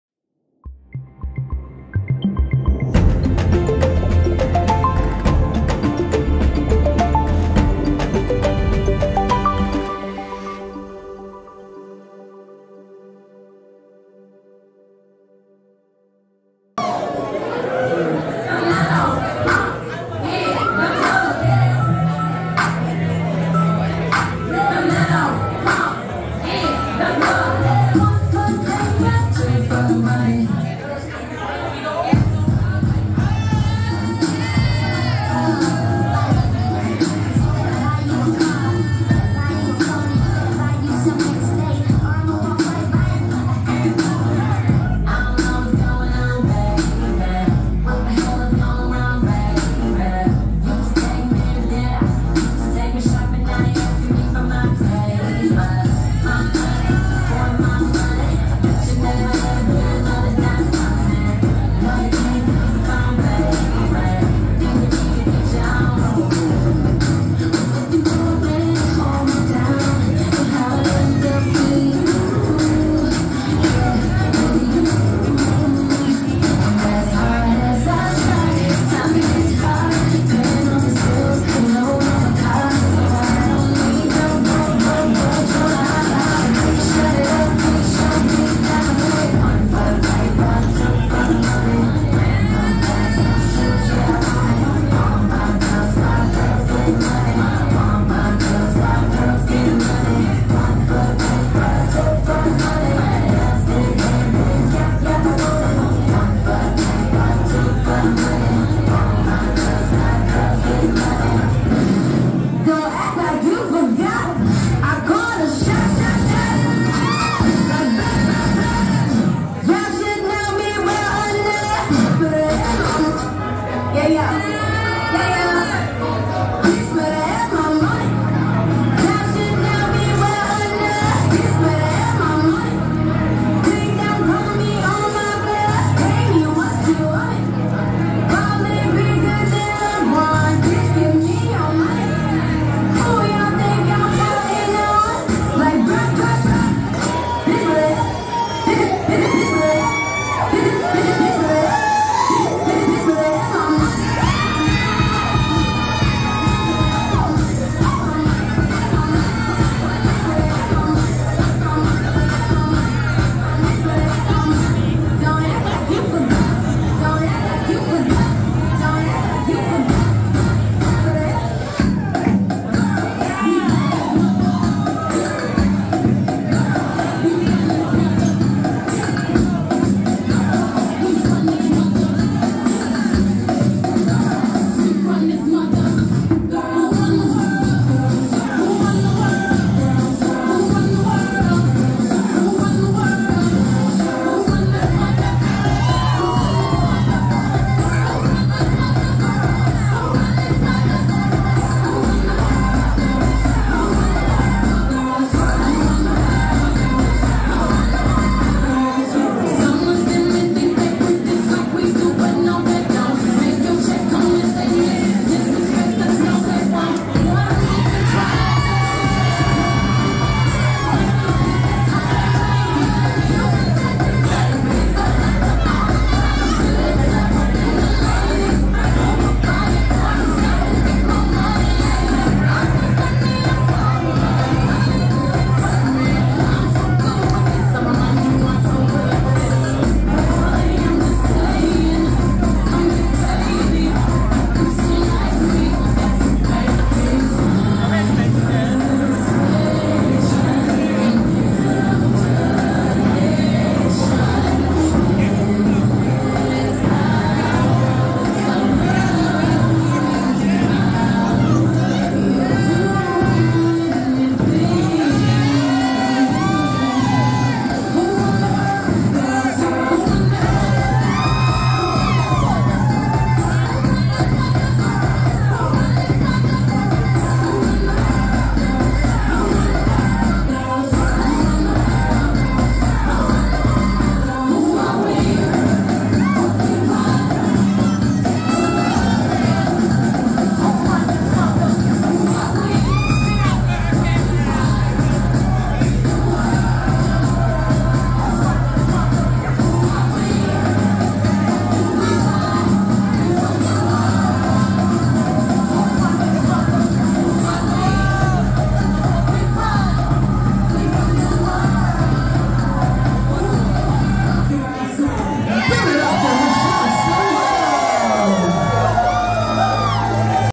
Monday Night Madness Finale: Michelle Stiletto
Special Guest: Michelle Stiletto!

Monday Night Madness is a weekly amateur competition.  It allows entertainers of all skill levels and styles the chance to perform in a real club setting.  A finalist is selected each week for six weeks and then a 7th Host's Pick is chosen.  These 7 contestants compete in a Finale.  The winner gets a 7 week booking in the show as the MNM Champion.